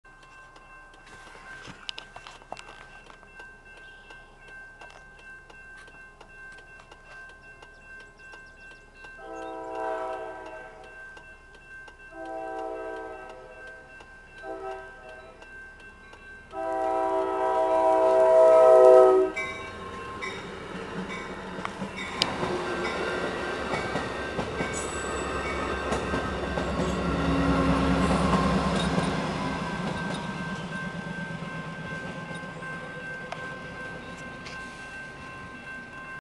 Amtrak phase III Heritage #406 on the Pere Marquette!
My dad took this one, we've been tracking it for the past two days, its been run from Chicago to Grand Rapids for the last two days.